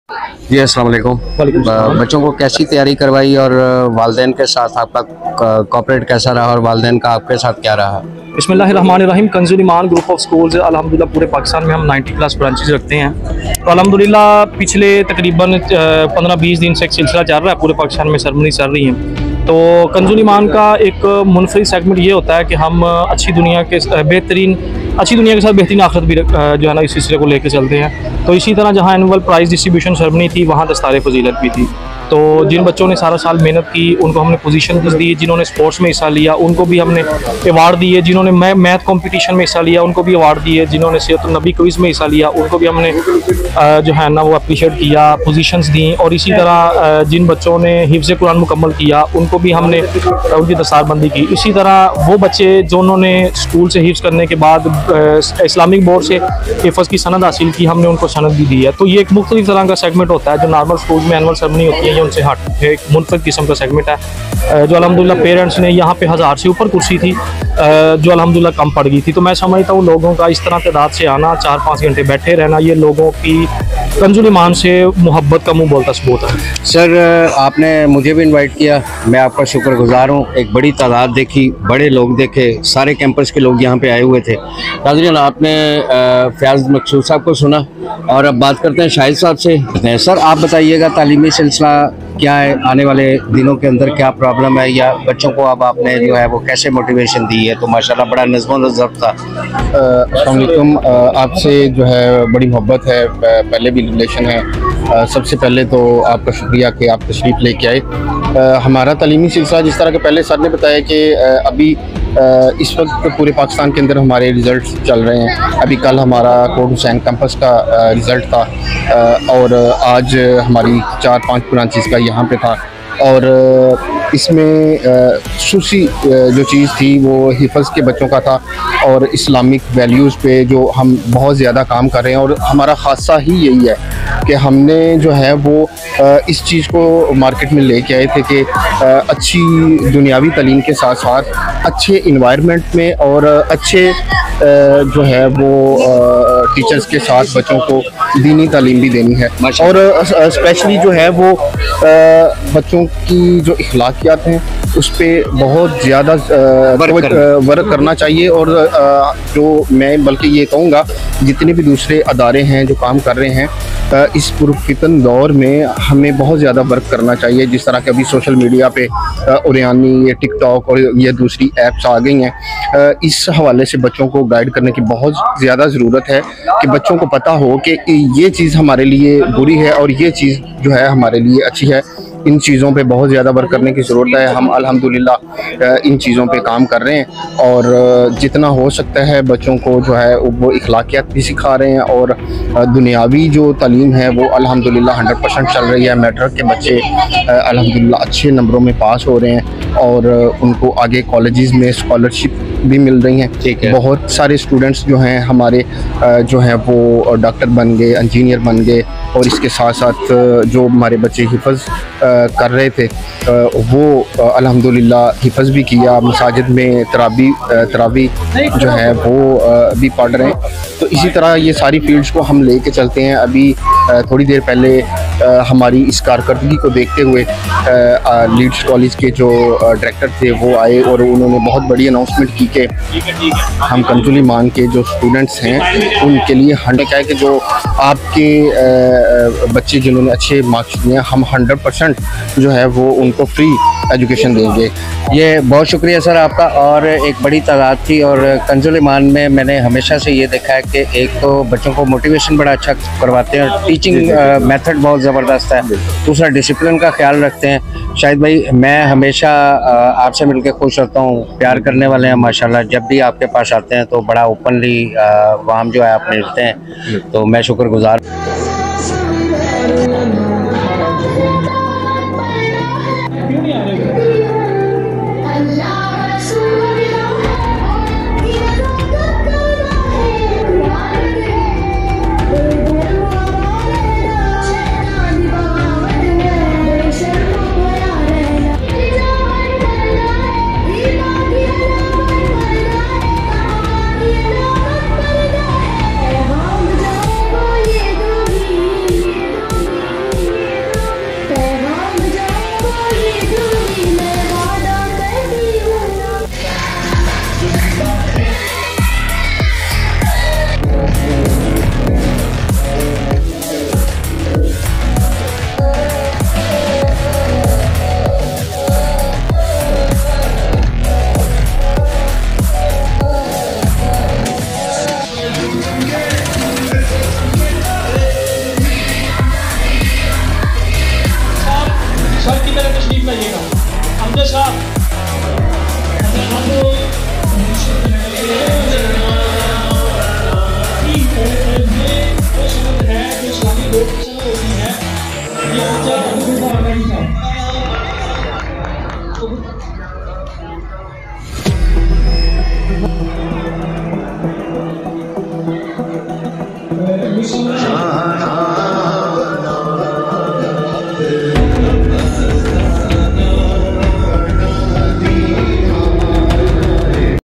KANZUL-IMAN||group of ||SCHOOLS||ANNUAL PRIZE DISTRIBUTION||CEREMONY
Dastar. E. Fazeelat||Hifz. E. Quran||Annual prize distribution||kanzul. Iman group of schools||Sheikhupura